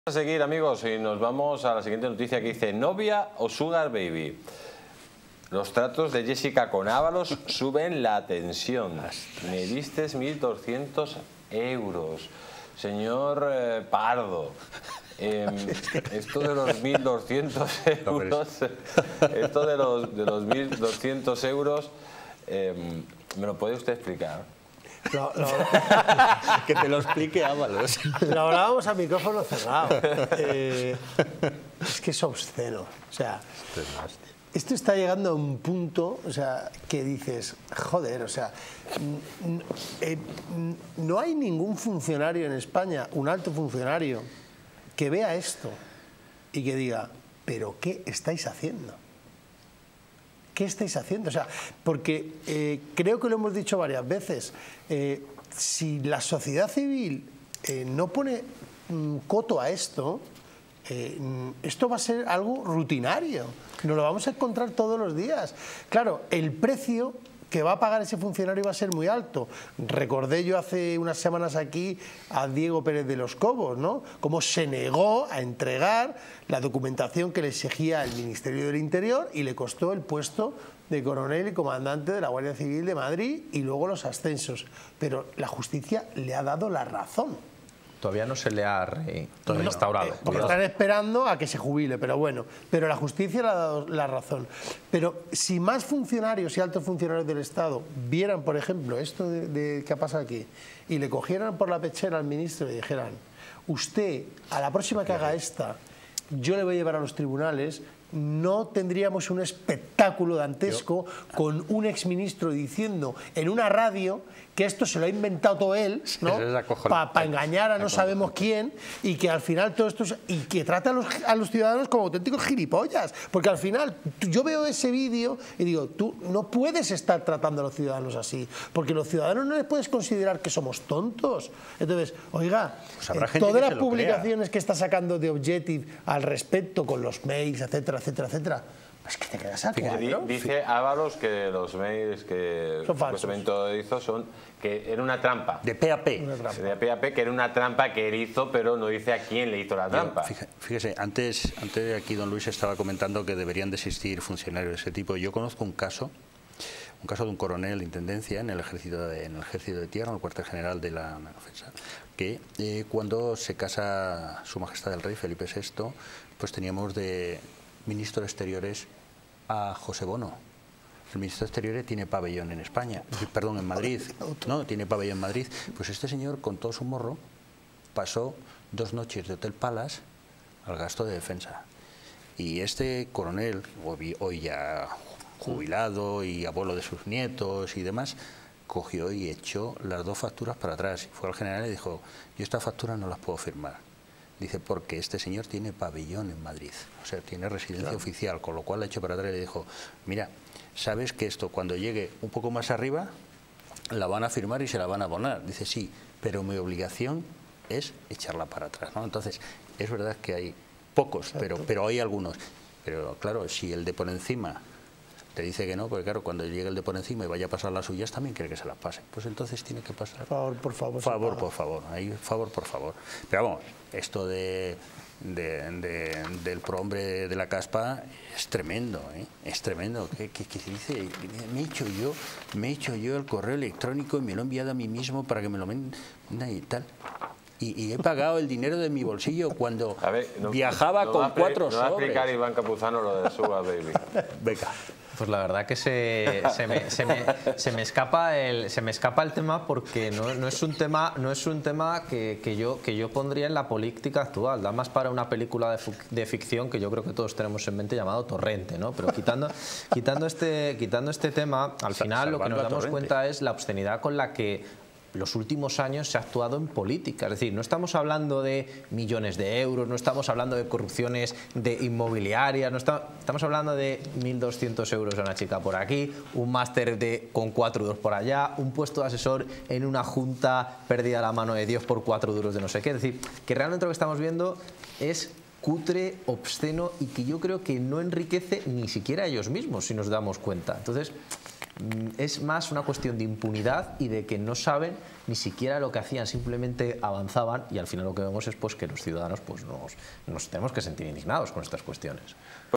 🔴¿Novia o sugarbaby?🔴 Los tratos de Jésica con Ábalos suben la tensión: “me diste 1.200 euros”
¡Descubre los tratos de Jésica con Ábalos en este video! La tensión sube cuando se revela que le dio 1,200 euros. ¡No te lo pierdas en Distrito TV!